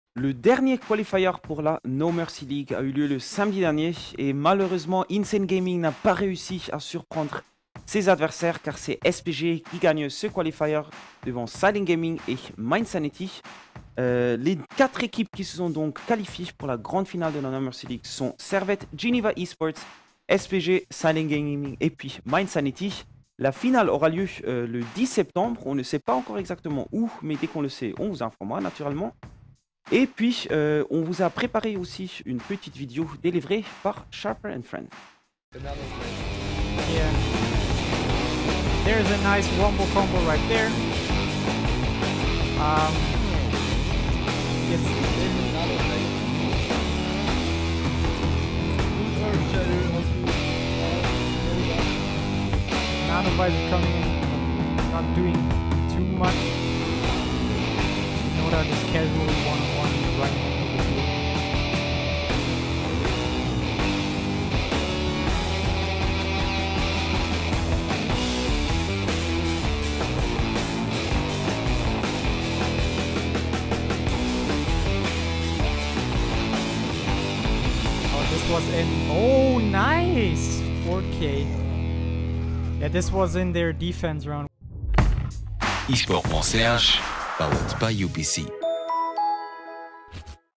Les 4 participants du tournoi final du No Mercy League sont connus - l'actu ligues et tournois
Servette Geneva eSports et SPG esports ont déjà assuré leurs places pour les phases finales de la No Mercy League. Quelles seront les deux équipes qui les rejoindront pour le tournoi final?